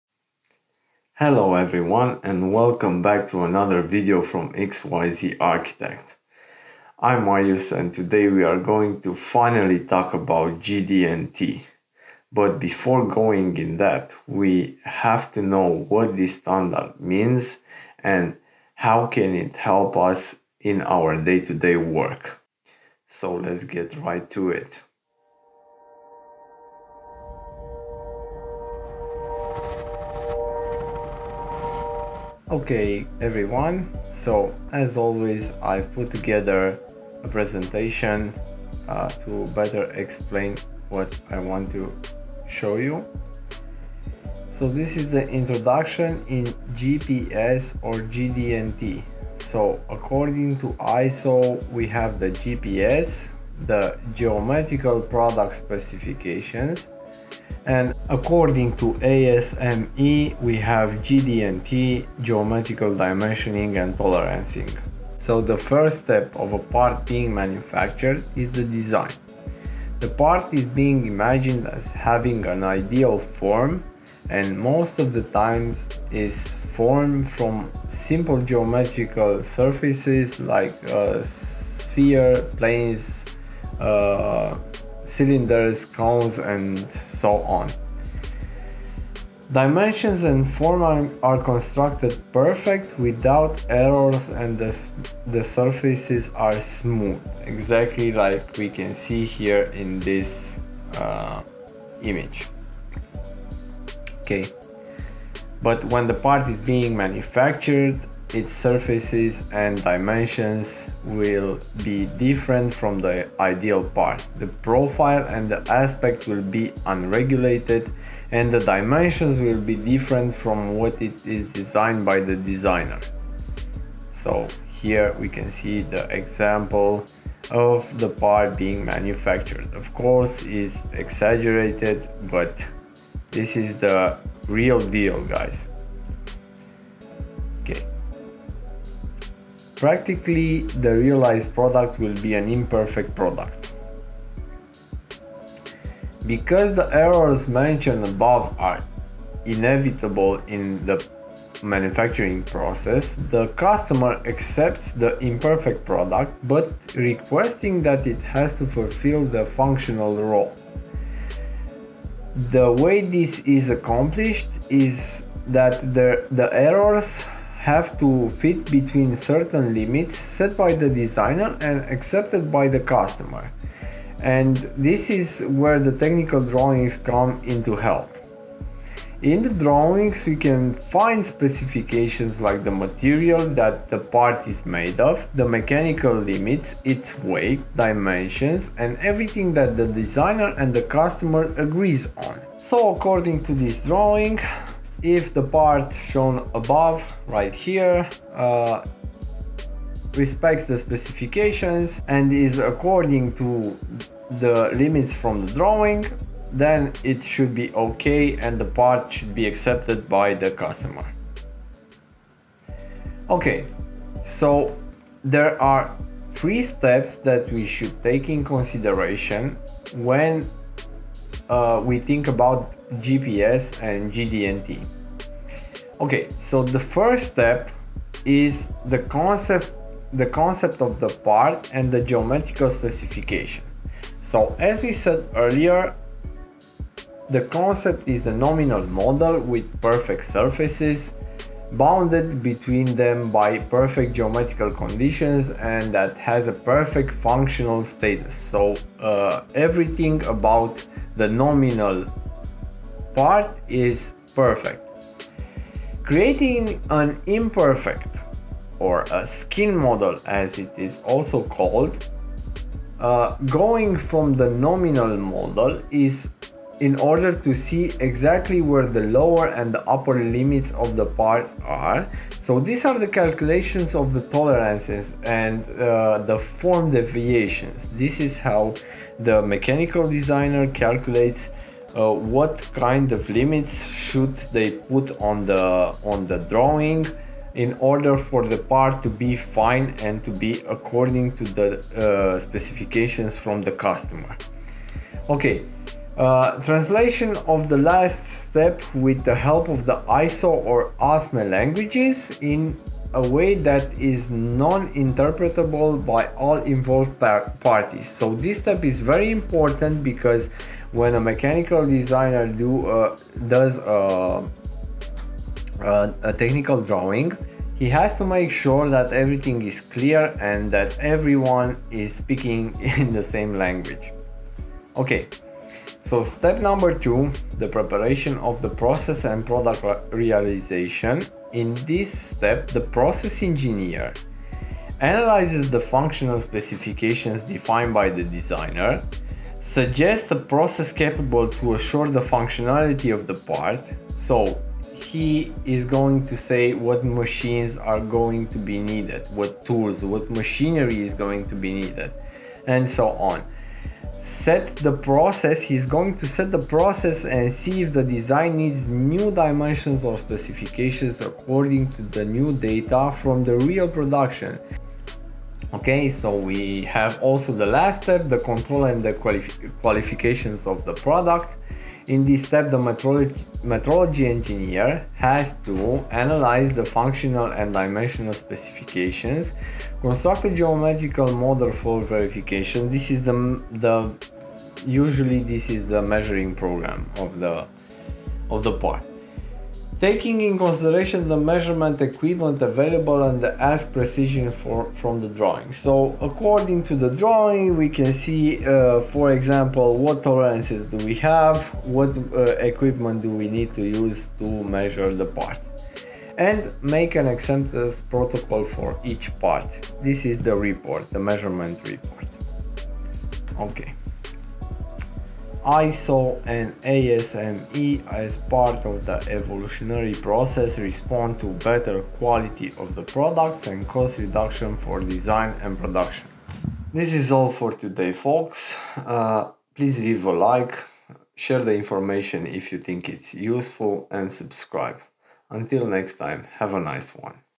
GPS and GD&T
In this video we talk about the introduction in GD&T and GPS. Enjoy the content!